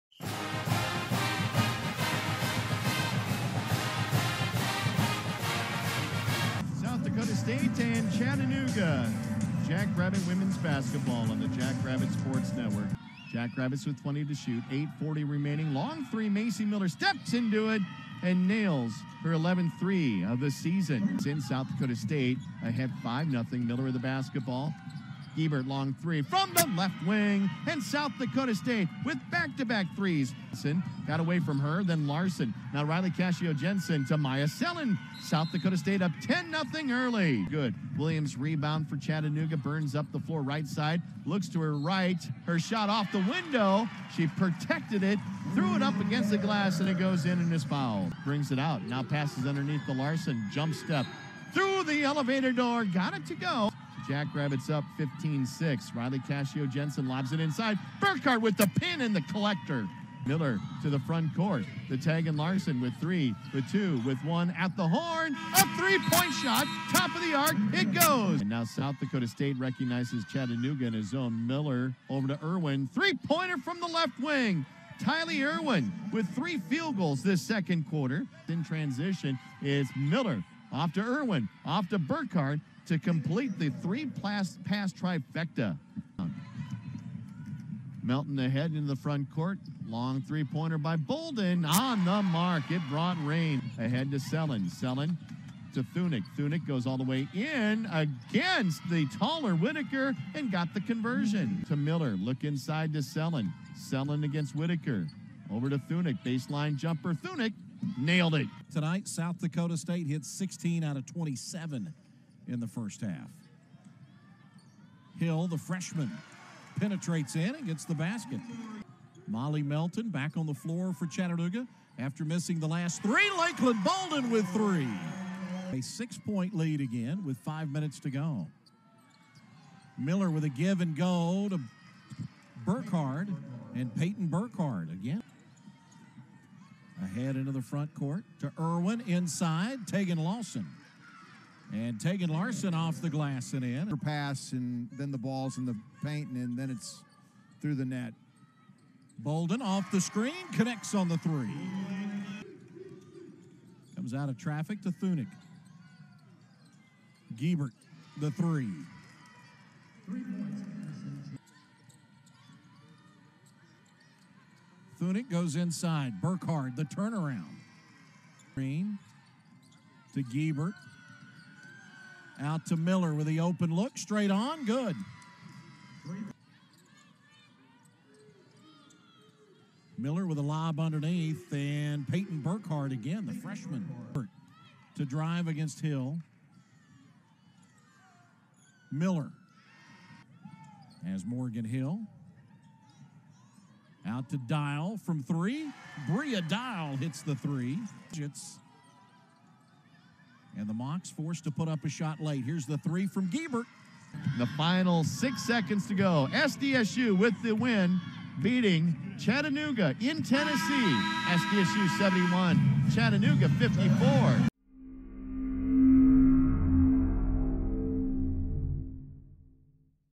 Women's Basketball at Chattanooga Highlights (12.04.2018)
The Jackrabbit women’s basketball team improved to 5-3 on the season after picking up a road win 71-54 over Chattanooga.